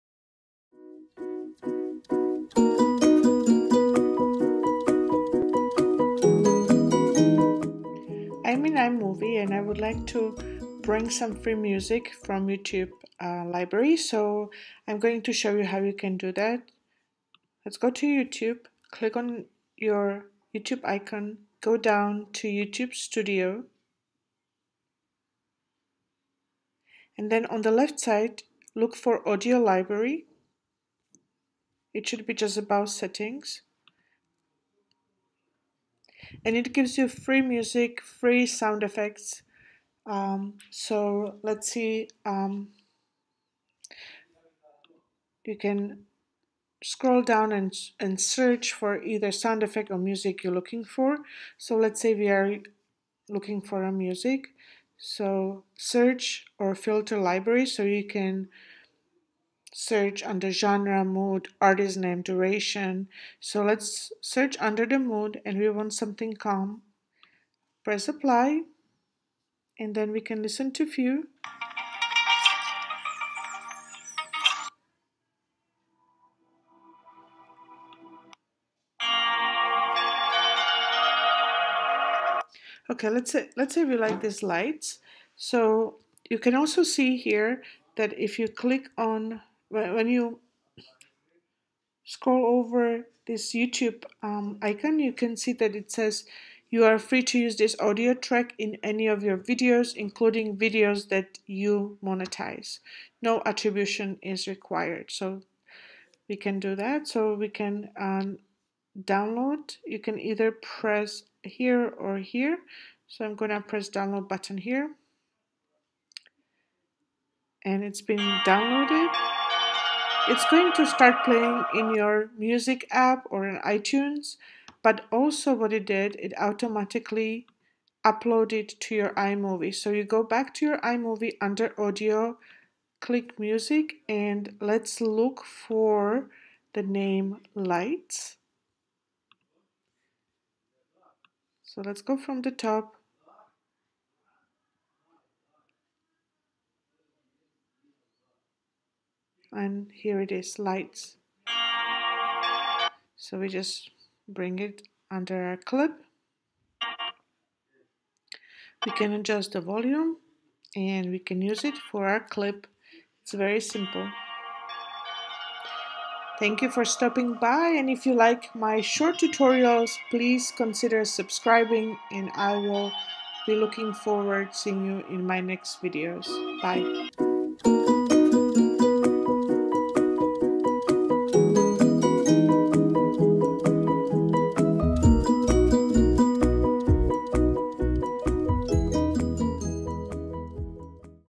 iMovie Tutorials - FREE Music & Sound Effects from YouTube to iMovie
Thanks for checking out Short Tutorials on YouTube!
How to upload MUSIC and SOUND EFFECTS from YouTube to iMovie:
* Click on your account icon (Top Left)
* Click on YouTube Studio
* Choose music or sound effect you like and download (on the right side)
* Song or sound effects automatically uploads to your iMovie
* Look for the song in your iMovie
* Drag it under your clip - adjust the volume

My name is Eva and I gladly share short tutorials about software tips as I learn along the way.